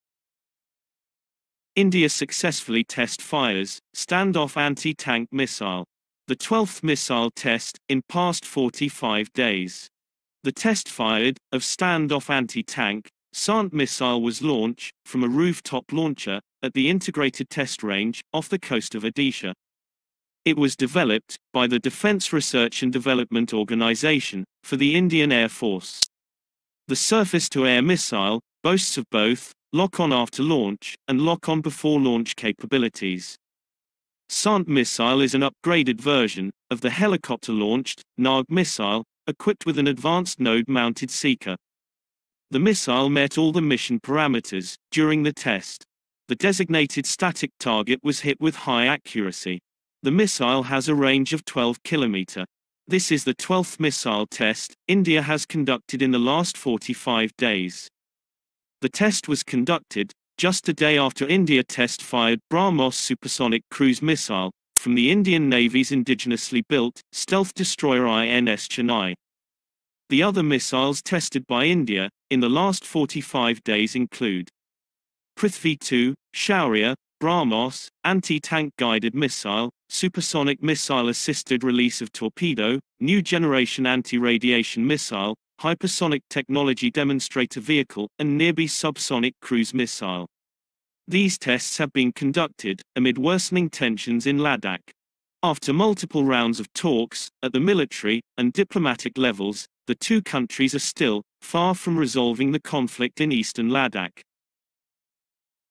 India Successfully Test Fires Stand Off Anti Tank Missile
India Successfully Test-Fires Stand-Off Anti-Tank Missile; 12th Missile Test In Past Forty-Five Days. India has successfully test-fired Stand-off Anti-tank (SANT) missile from a roof-top launcher at the Integrated Test Range (ITR) off the coast of Odisha,
Developed by the Defence Research and Development Organisation (DRDO) for the Indian Air Force (IAF), the surface-to-air missile boasts of both, Lock-on After Launch and Lock-on Before Launch capabilities
SANT missile is an upgraded version of the Helicopter Launched Nag (HeliNa) missile, equipped with an advanced node-mounted seeker.

The missile met all the mission parameters during the test,The designated static target was hit with high accuracy,The missile has a range of 12 kilometer,This is the twelfth missile test India has conducted in the last 45 days.

The test was conducted just a day after India test-fired BrahMos supersonic cruise missile from the Indian Navy's indigenously-built stealth destroyer INS Chennai.

The other missiles tested by India in the last 45 days include Prithvi-II, Shaurya, BrahMos, Anti-Tank Guided Missile, Supersonic Missile Assisted Release of Torpedo, New Generation Anti-Radiation Missile, Hypersonic Technology Demonstrator Vehicle and Nirbhay subsonic cruise missile,These tests have been conducted amid worsening tensions in Ladakh.

After multiple rounds of talks at the military and diplomatic levels, the two countries are still far from resolving the conflict in eastern Ladakh